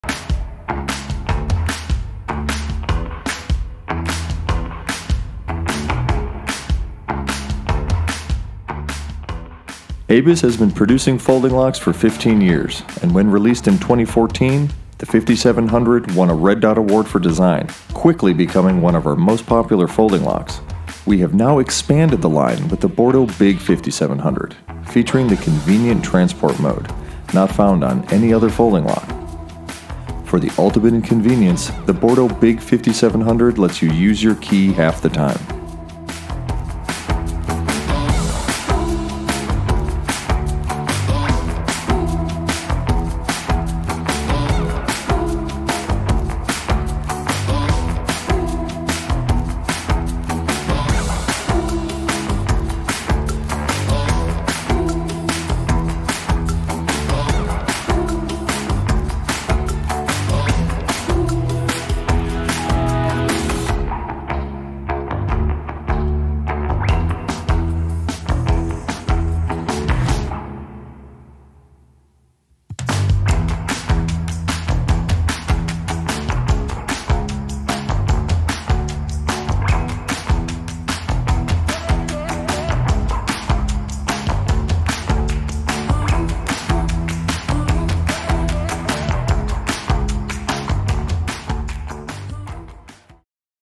ABUS Bordo uGrip 5700 Folding lock
The ABUS Bordo uGrip 5700 Big is now available.  Use your key half the time thanks to the Convenient Transport Mode (CTM)™.